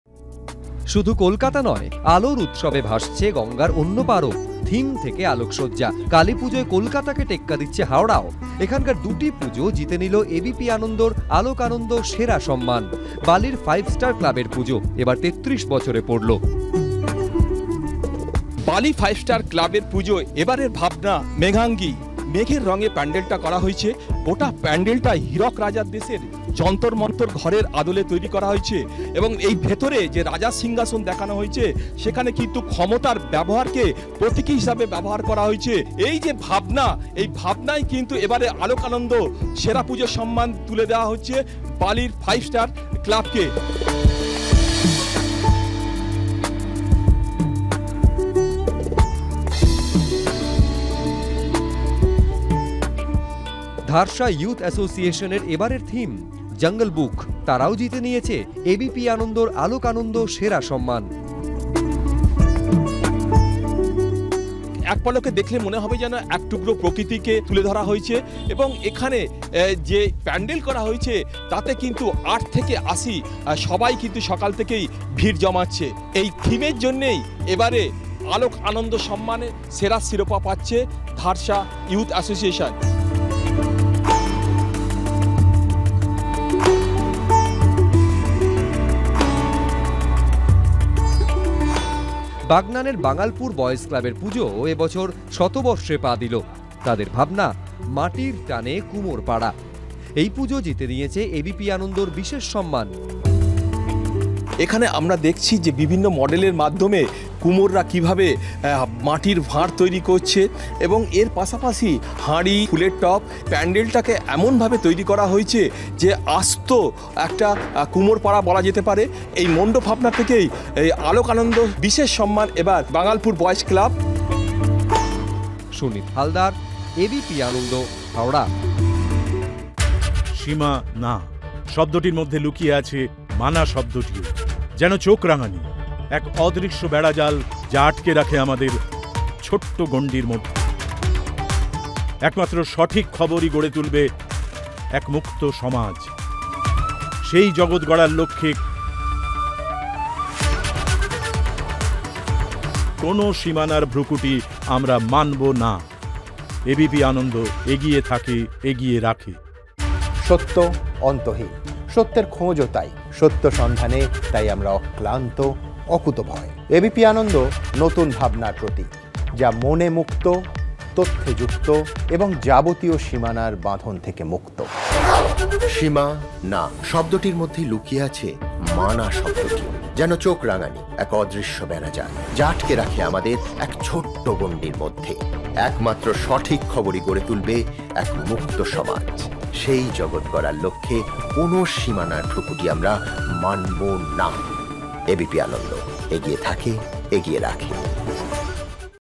Kali Puja 2022:অভিনব থিম, চোখ ধাঁধানো আলোকসজ্জা, আলোক আনন্দ সেরা সম্মান হাওড়ার দুটি পুজোর
থিমে অভিনব চিন্তাভাবনা থেকে চোখ ধাঁধানো আলোকসজ্জা। কালীপুজোয়  এবিপি আনন্দ-র আলোক আনন্দ সেরা সম্মান জিতে নিল হাওড়ার দুটি পুজো। বিশেষ সম্মান পেয়েছে একটি পুজো।